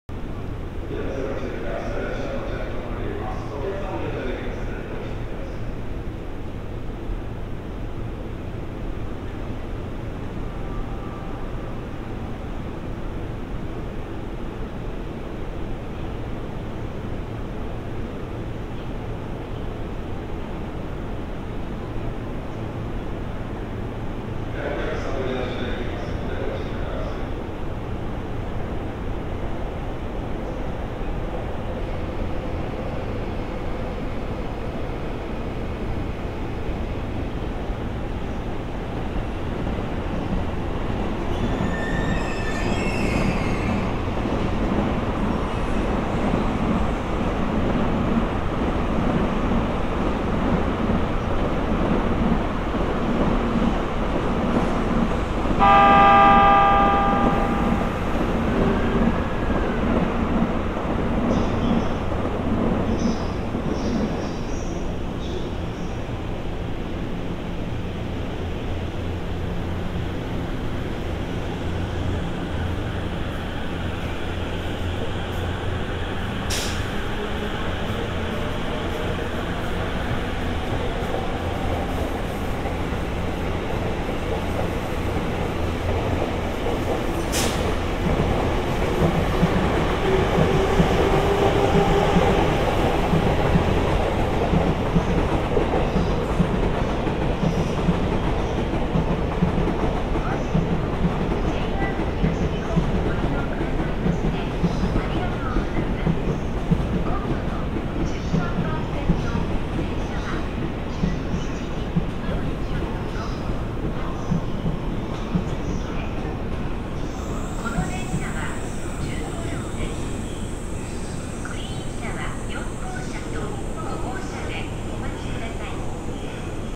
2019/10/01 【東海道本線試運転】 E257系 NA-03編成 品川駅 | JR East: Test Run of E257 Series NA-03 Set at Shinagawa
JR東日本
東海道本線
試9531M
東京 ～ 伊東
E257系 NA-03編成
品川駅
2019年10月1日